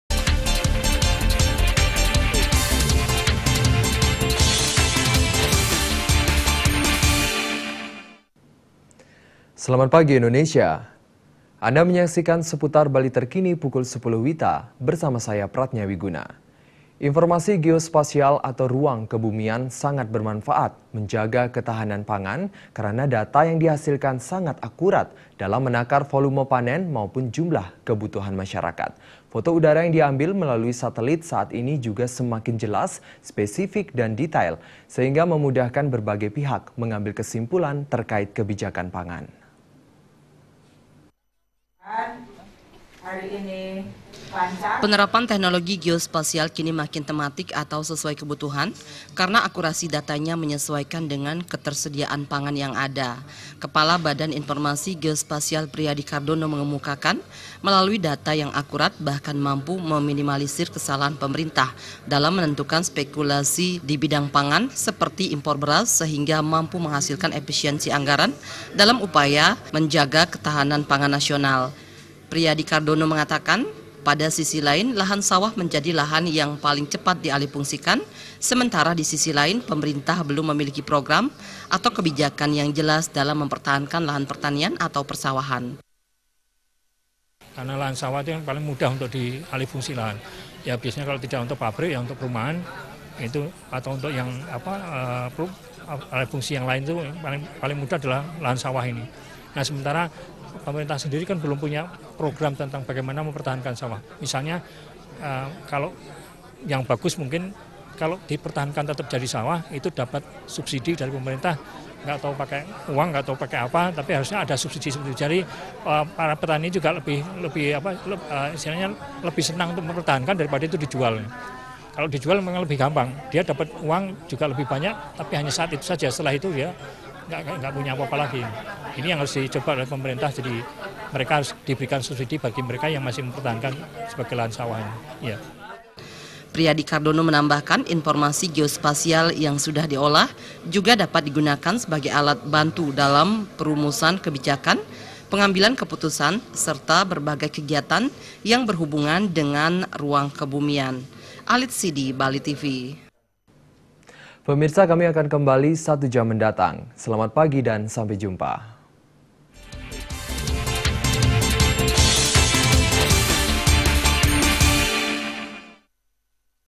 INFORMASI GEOSPASIAL JAGA KETAHANAN PANGAN
INFORMASI GEOSPASIAL ATAU RUANG KEBUMIAN, SANGAT BERMANFAAT MENJAGA KETAHANAN PANGAN, KARENA DATA YANG DIHASILKAN SANGAT AKURAT. DALAM MENAKAR VOLUME PANEN MAUPUN JUMLAH KEBUTUHAN MASYARAKAT. FOTO UDARA YANG DIAMBIL MELALUI SATELIT SAAT INI JUGA SEMAKIN JELAS, SPESIFIK, DAN DETAIL, SEHINGGA MEMUDAHKAN BERBAGAI PIHAK MENGAMBIL KESIMPULAN TERKAIT KEBIJAKAN PANGAN.